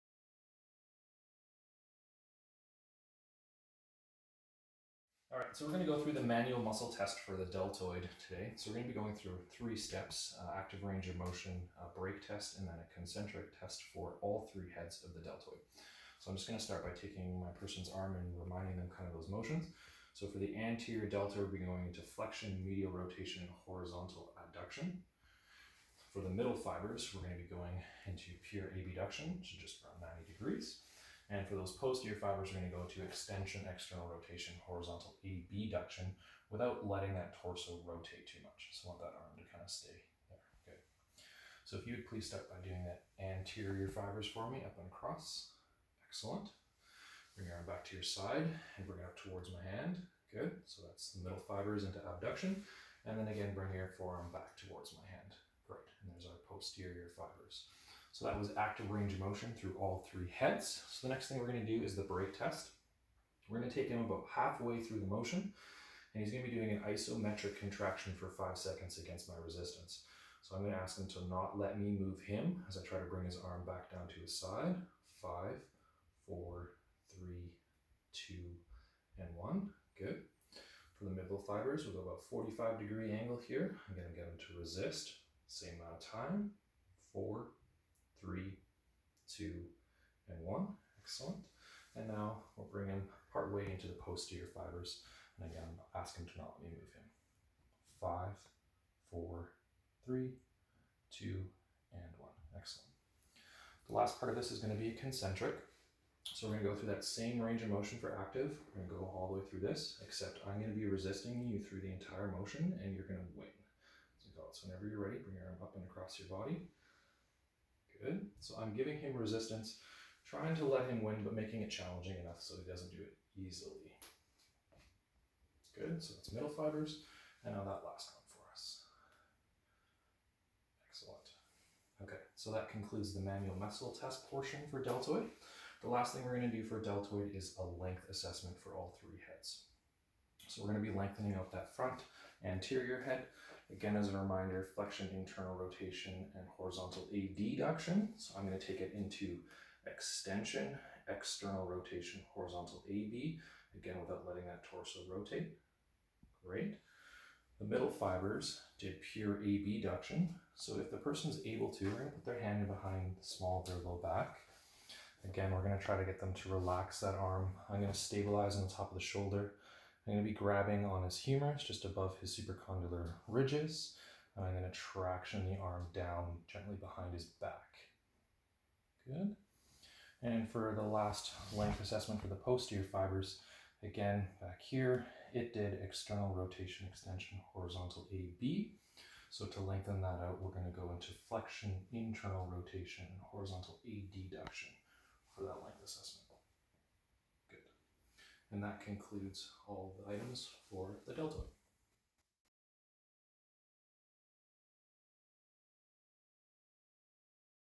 Manual Muscle Test & Lengthening - Deltoid
This is a detailed explanation of how to manual muscle test and lengthen deltoid muscle by Keith Bootsma, RMT.

Activation:
Anterior Fibers: Flexion of the Arm
Middle Fibers: Abduction of the Arm
Posterior Fibers: Extension of the Arm

Lengthening:
Anterior Fibers: Extension of the Arm
Middle Fibers: Adduction of the Arm
Posterior Fibers: Flexion of the Arm